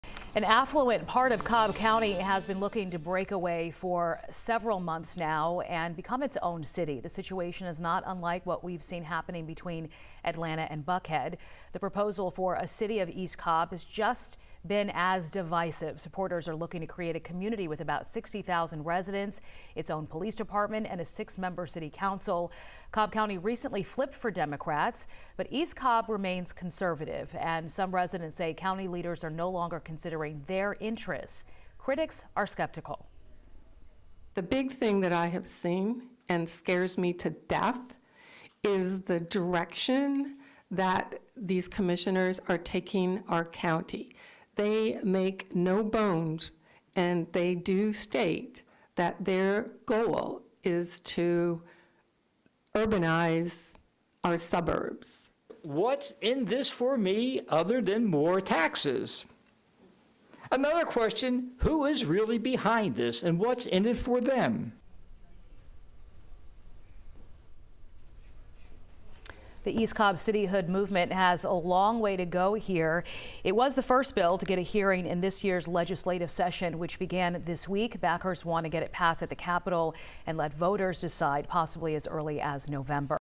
Proposed city of East Cobb faces criticism
An affluent part of Cobb County has been looking to break away from months now.